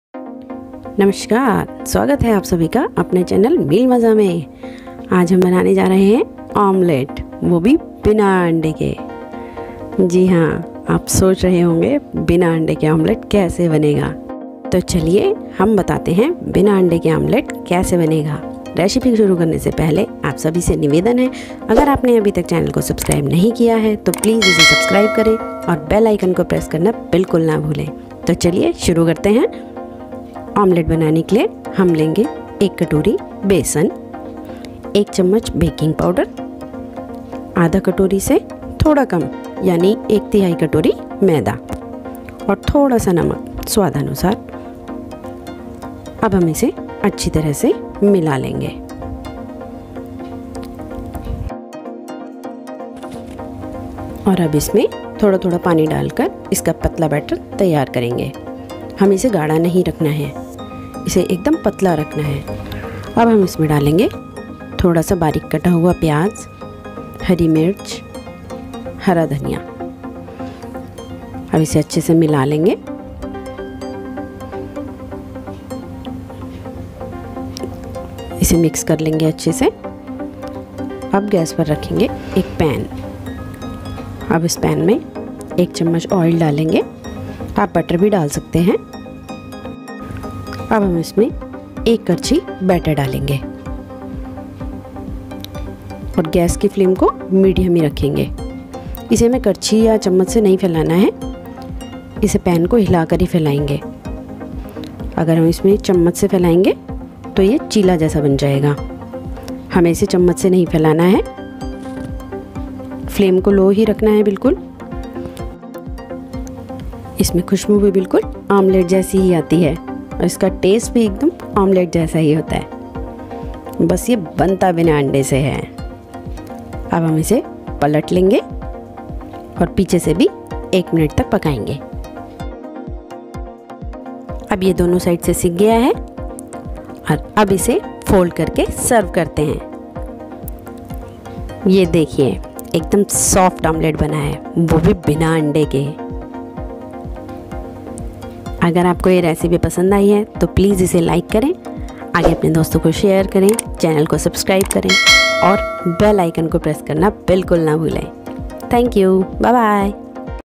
बिना अंडे का ऑमलेट ।। Bina Andey Ka Omelette Banaye Aasani Se
Ingredients:

Besan  - 1cup
Baking Powder - 1tbsp
Onion ,Coriander leaves, Green Chilli

आवश्यक सामग्री :

बेसन   - 1 कटोरी
मैदा     - 1/3 कटोरी
नामक  - 1 चम्मच
बेकिंग पाउडर - 1 चम्मच
प्याज, हरा धनिया, हरि मिर्च - स्वादानुसार